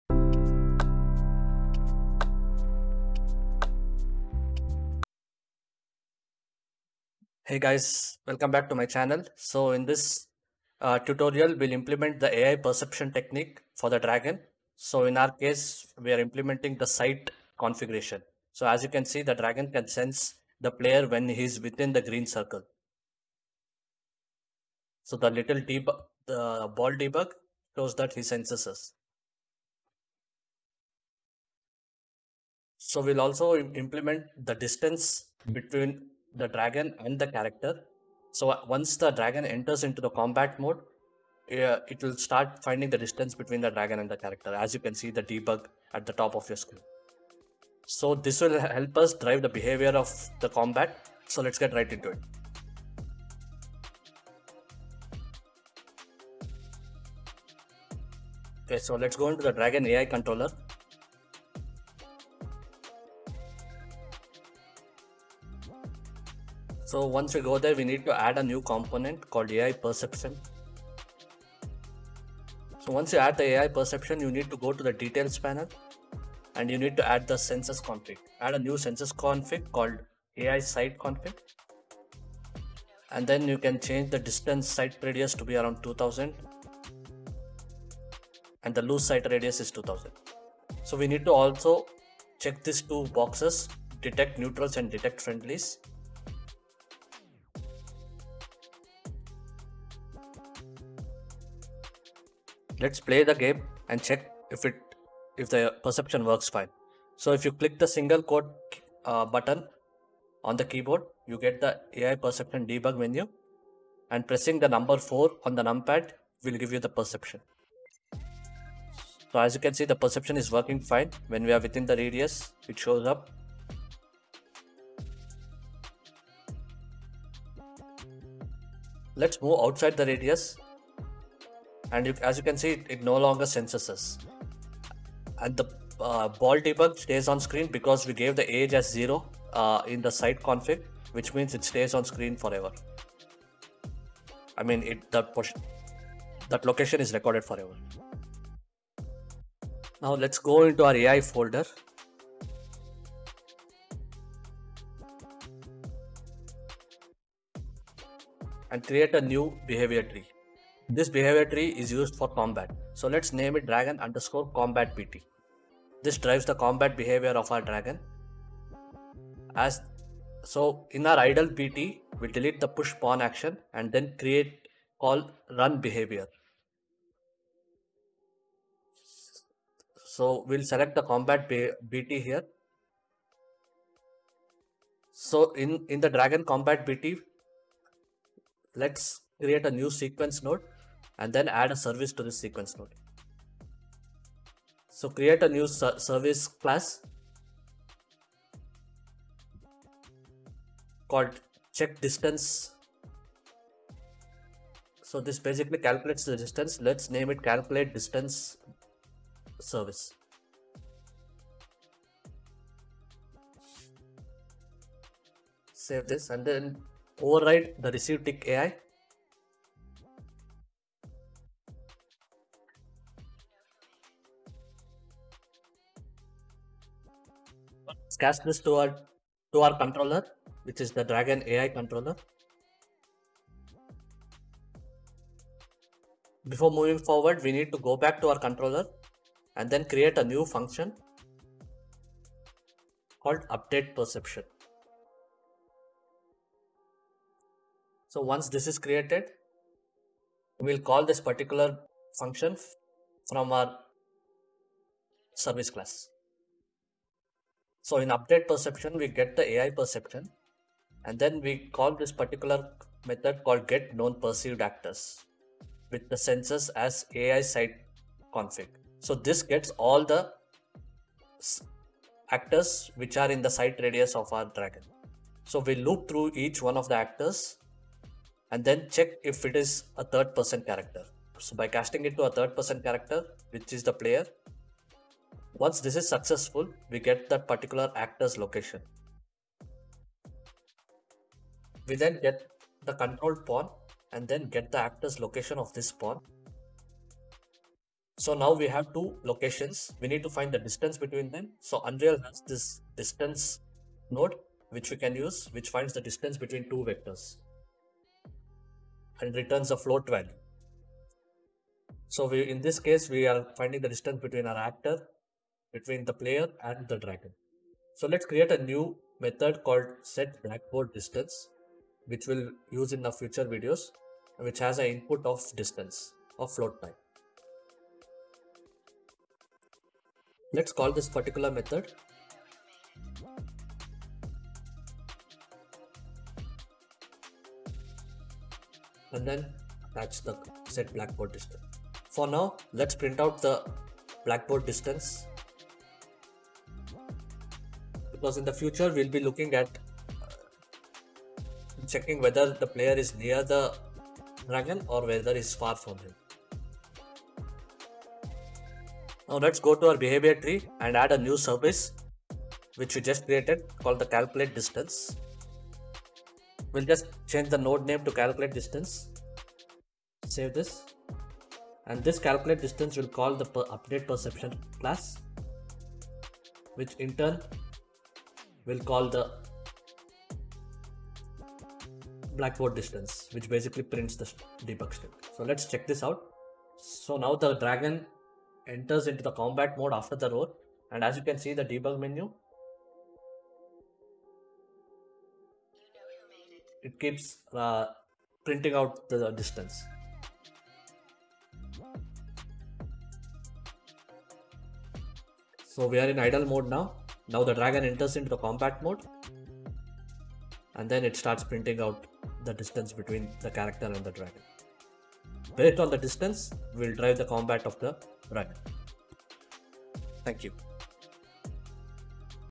Dragon vs Archer #4 | Unreal's AI Perception and calculating distance between player and dragon
In this video, we implement unreal's AI perception for sensing the player in the world and using that technique we find the distance between the player and the dragon.

00:00 Introduction
00:53 AI perception component
02:28 Setting combat behavior tree
03:45 Calculate distance service
06:22 Outro